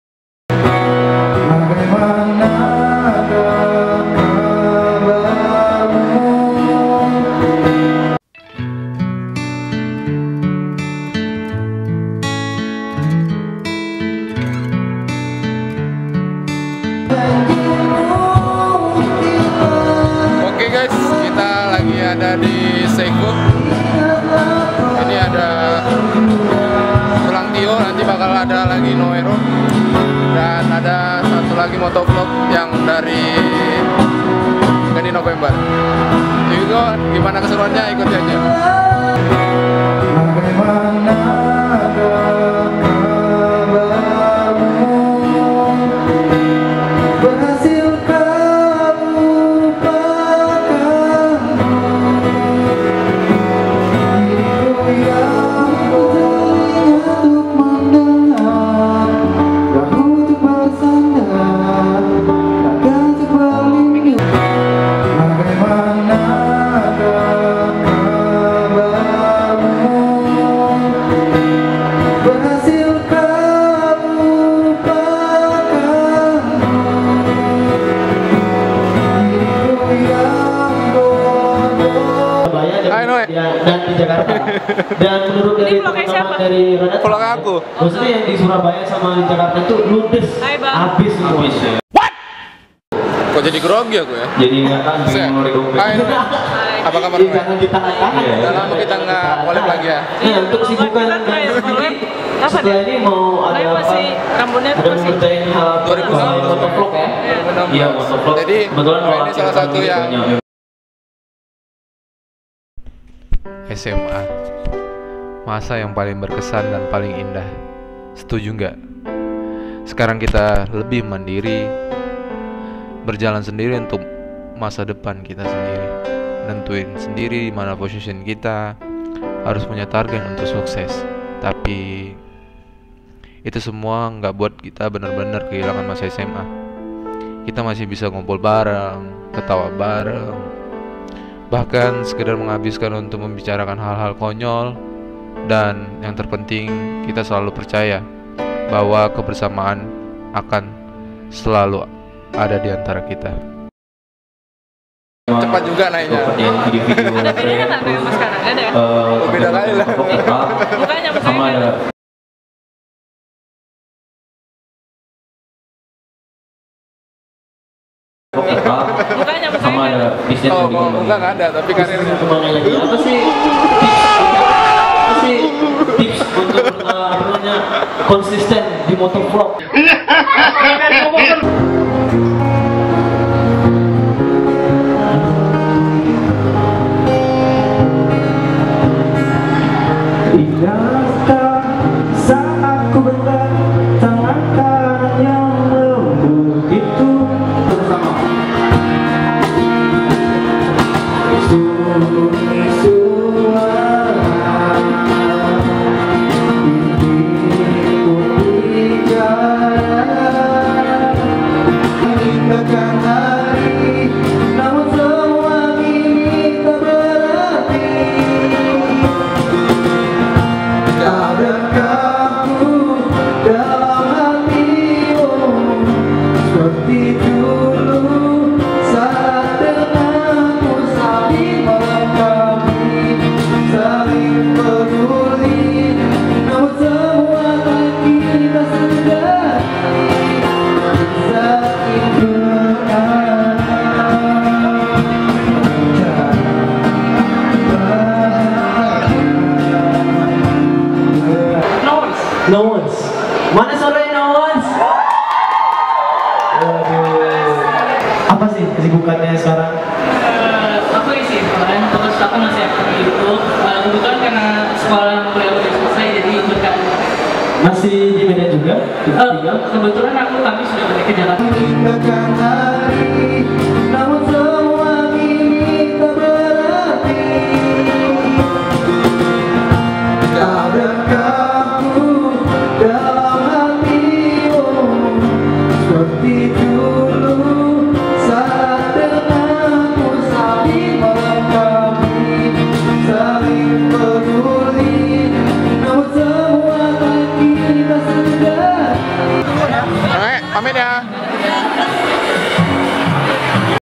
KETEMU NOE ROW UNTUK KEDUA KALINYA SETELAH 3 TAHUN ❗️❗️bareng tulangtio & ghandynovember
hai temen temen semua yang suka sama noe , makasih yang udah nonton , yang mau salam nanti di sampein kalau ketemu lagi.

😭😭 ada grogi groginya
@Noe Row